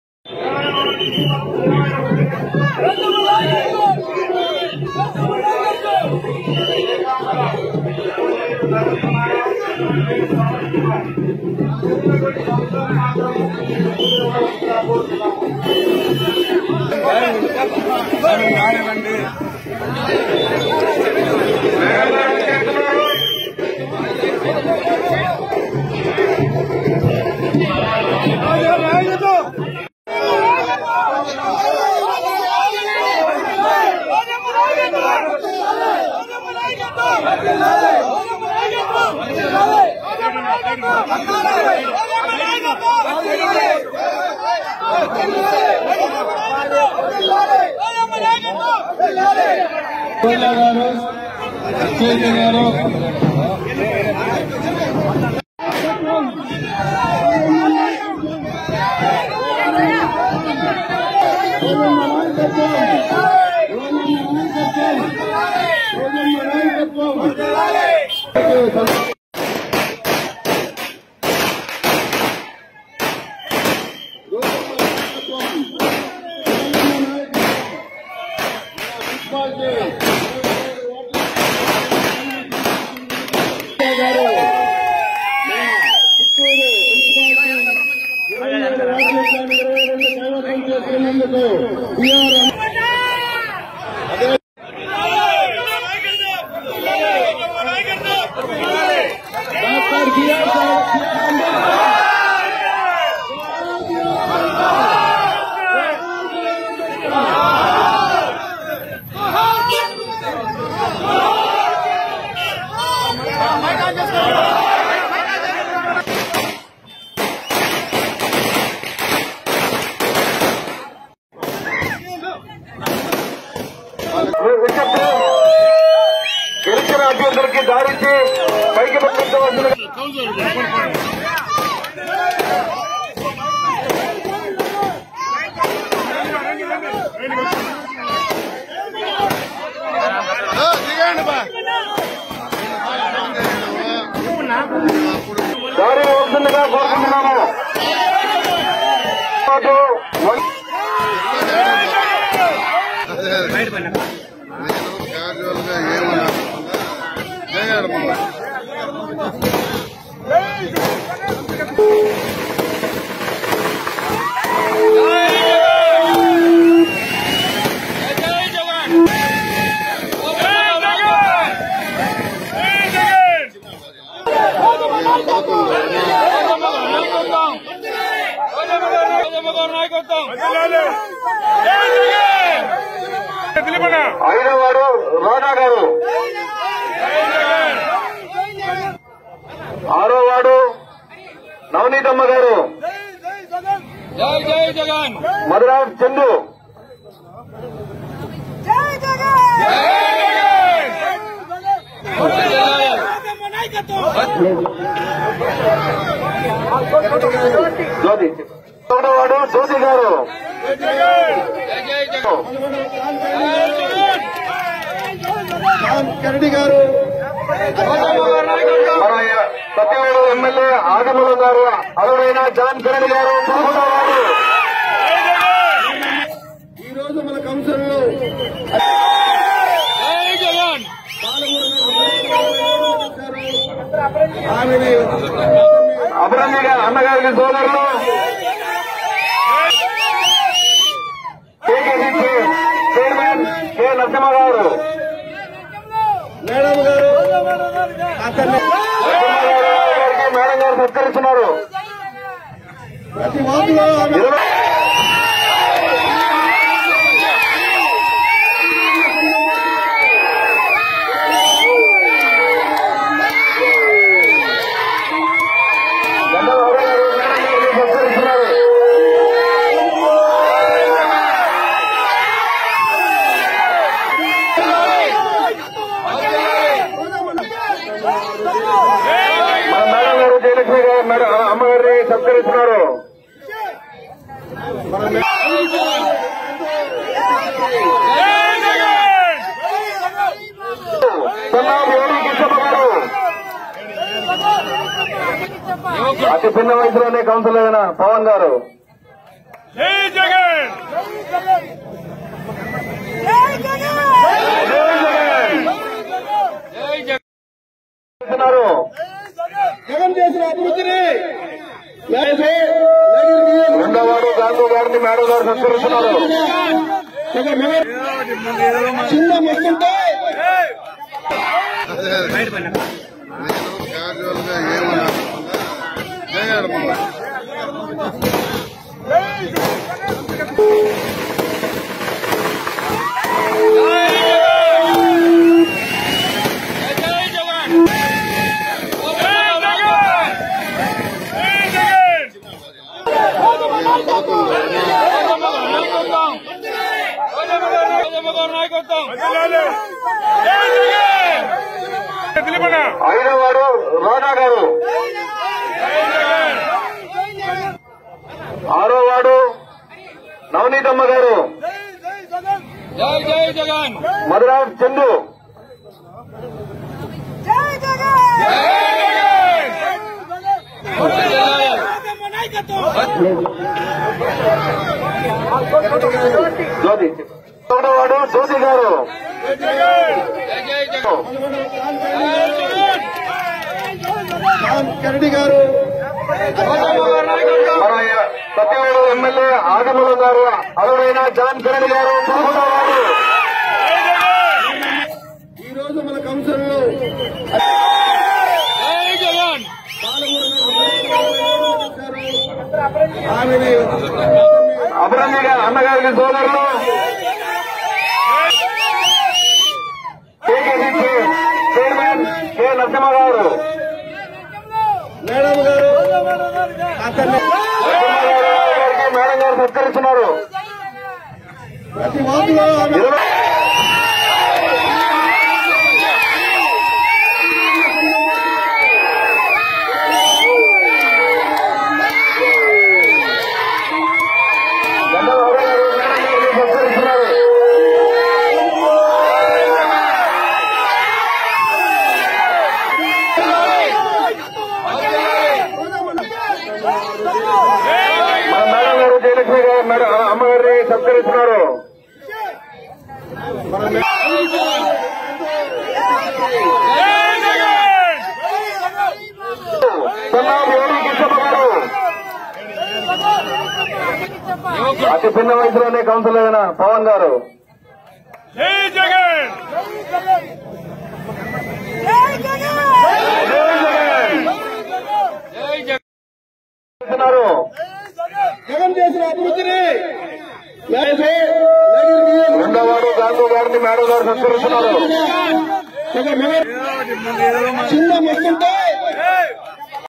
MLA Roja Celebrations | Ys Jagan | నగరి ఎమ్మెల్యే అడ్డాలో జగన్ పేరు మారిమోగిపోయింది | PDTV News
నగరి ఎమ్మెల్యే అడ్డాలో జగన్ పేరు మారిమోగిపోయింది